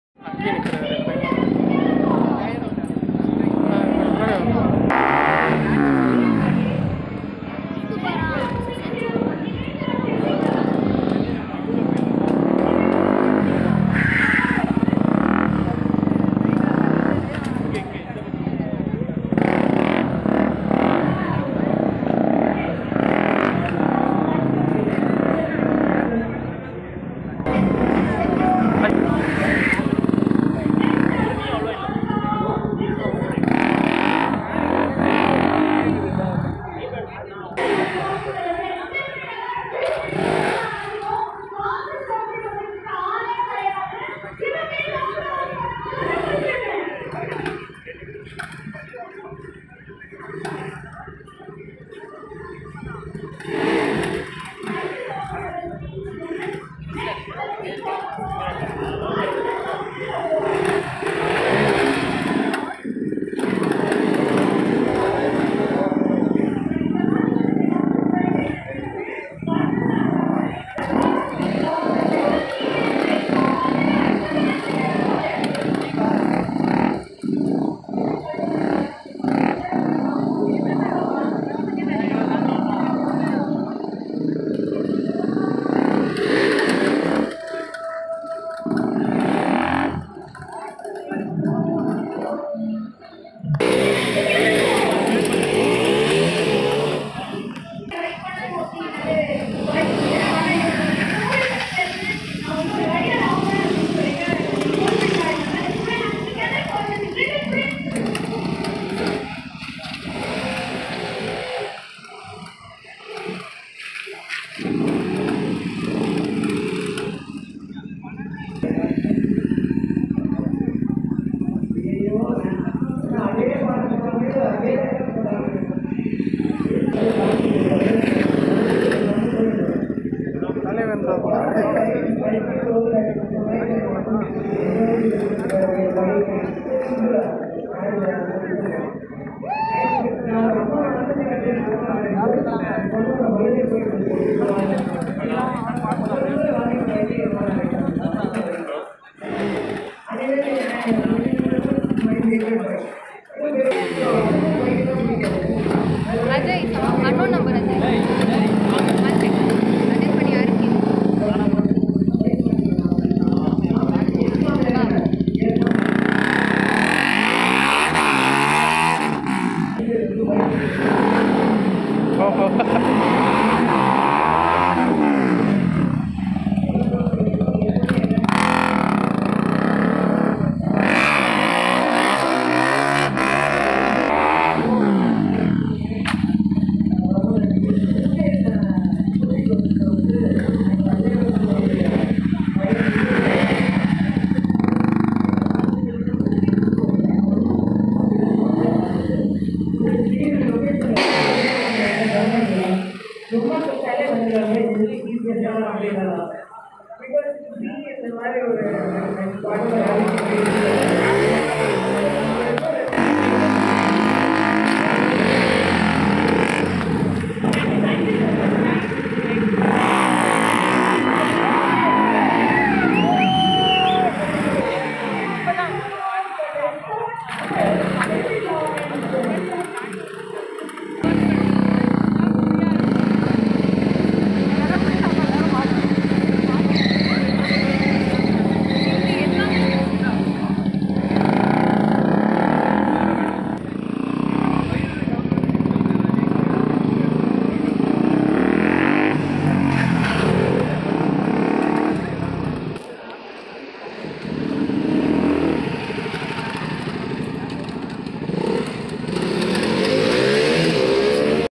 KTM STUNT SHOW BY DFG STUNT GROUP | KTM 200#myzonevlogs
Ktm stunt show by dfg stunt group with Ktm 200